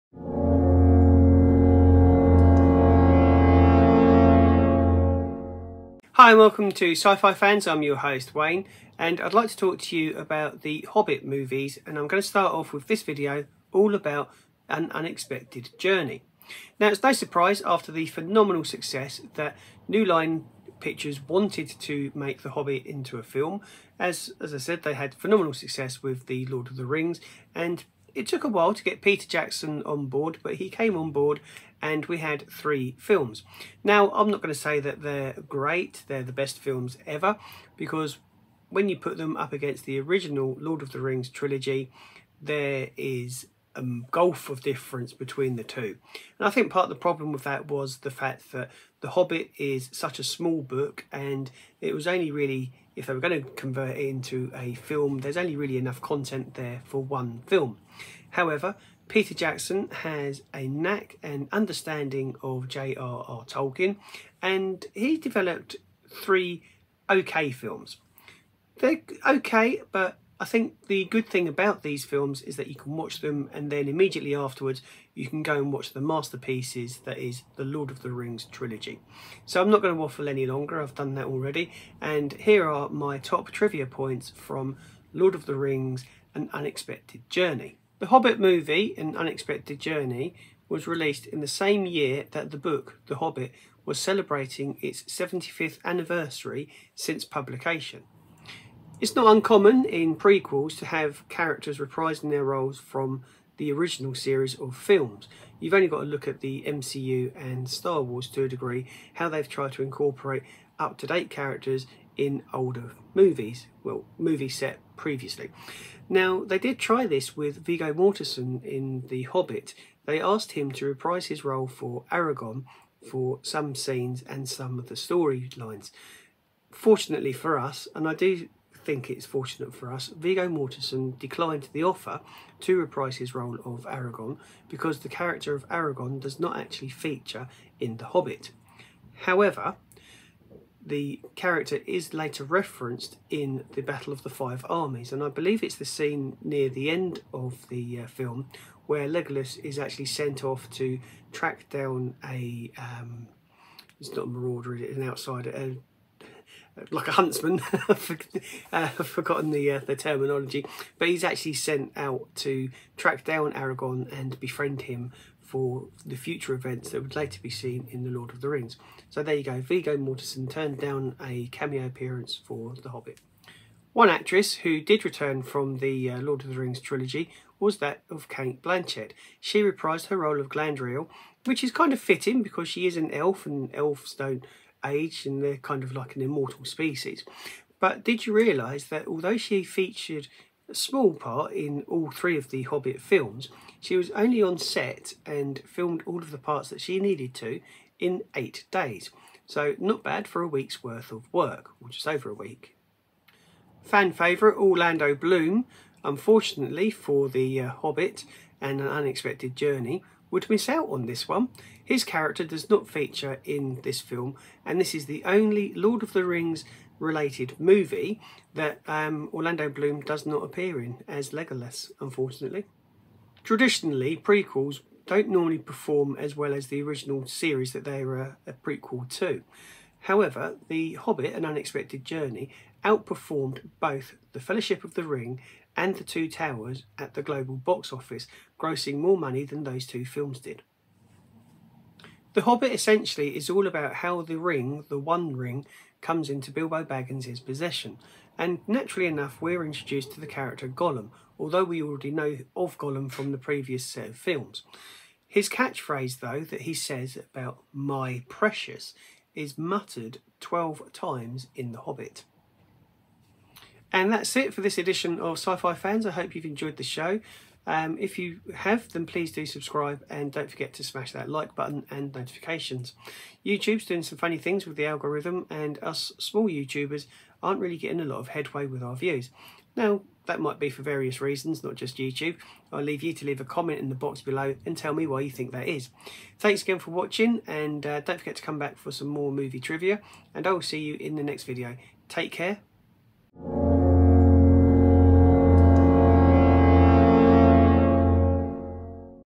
the hobbit movie trivia
Peter Jackson created the best trilogy of films ever. but could he do it again with the Hobbit? here are some things you didn't know about the first prequel film set in middle earth.